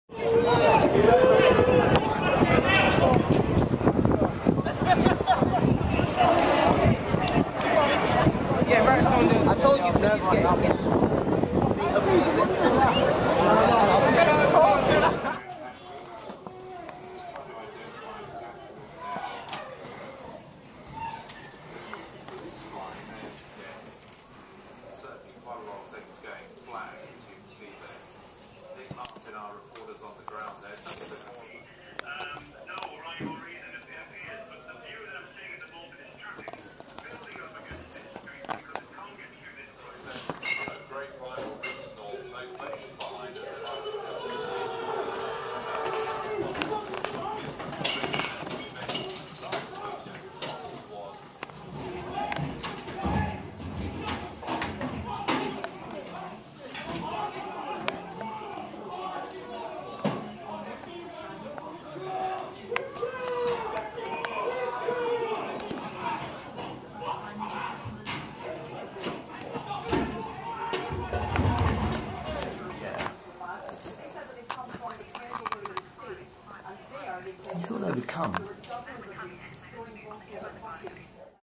riot in lewisham
police stand off with rioters in lewisham

NAME: rob
LOCATION RECORDED: lewisham